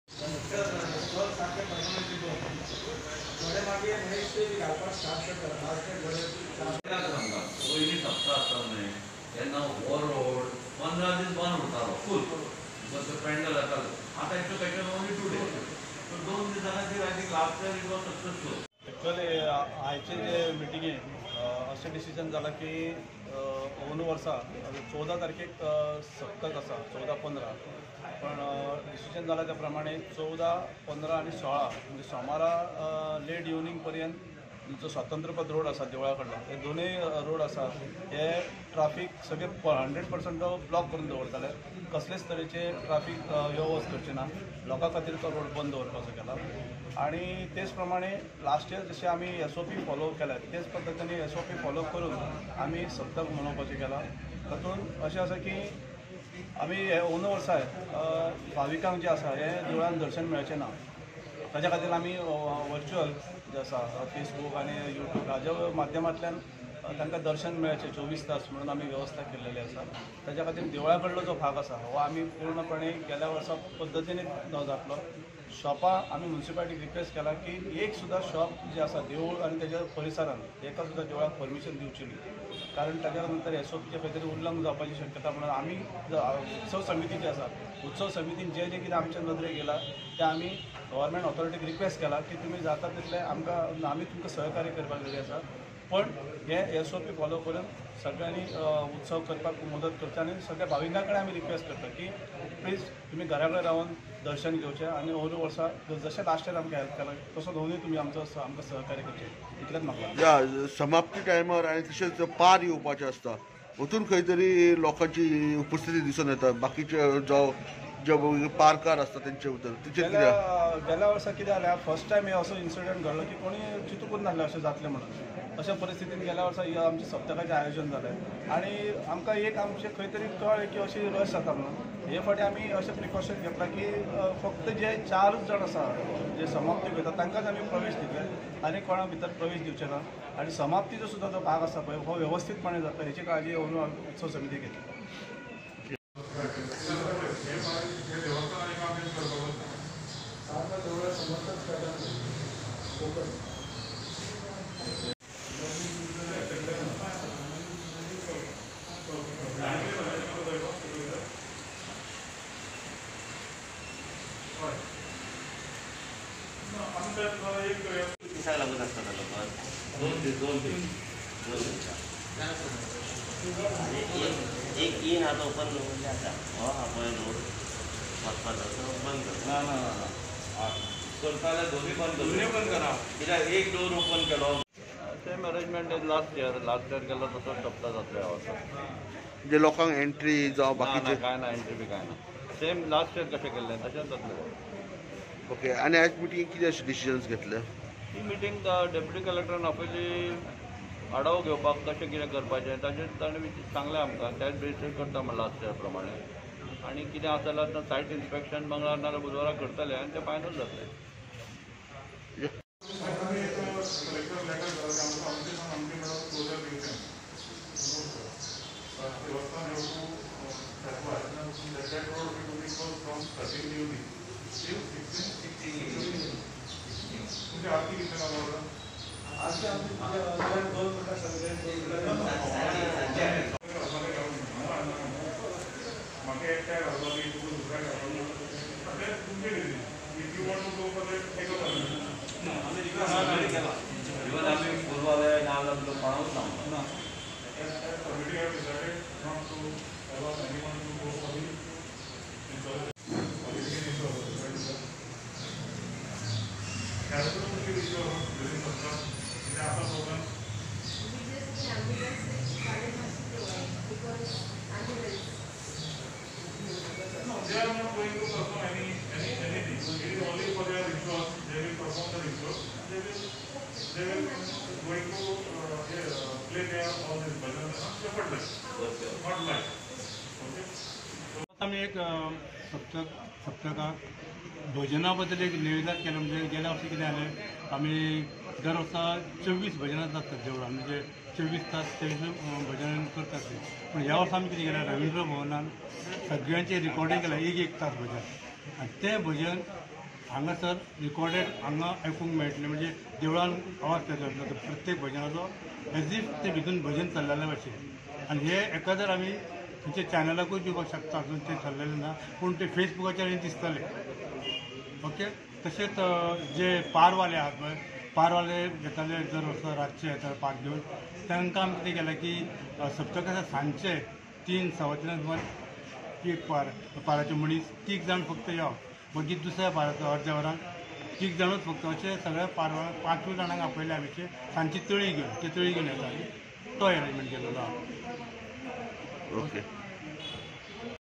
Saptah to be celebrated in subdued manner, in line with previous year
The much awaited Damodar Bhajani Saptah festival will be celebrated in a subdued manner ,second year in a row amidst the pandemic .
The Saptah organising commitee and Prashant Joshi of the Joshi Family on Friday evening  were present at a meeting convenened by Dy Collector to assess the arrangements to be made for Saptah Festival.
Speaking to media later, Santosh Khorjuvekar of the Saptah Organising commitee said that they had requested the Municipality not to allow any stalls to be alloted  for the Saptah Festival
" We will keep the roads of Swatantra Path blocked from 14th to 16th during the saptah and area around the temple will be barricaded " said Khorjuwekar
Prashant Joshi said that saptah was to be celebrated in same lines as last year.